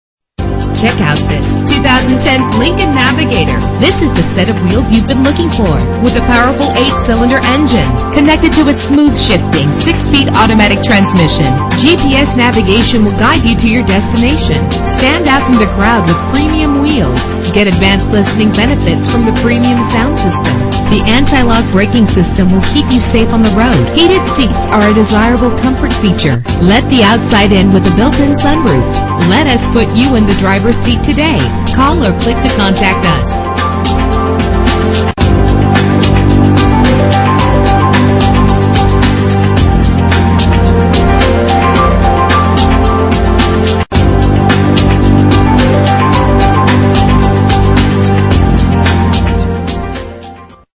2010 Lincoln Navigator - Norman OK
Year: 2010
Make: Lincoln
Model: Navigator
Trim: Base
Engine: 5.4 liter 8 cylinder 24 valve
Transmission: 6-Speed Automatic
Color: Black
Mileage: 29205
Address:
821 N. Interstate Drive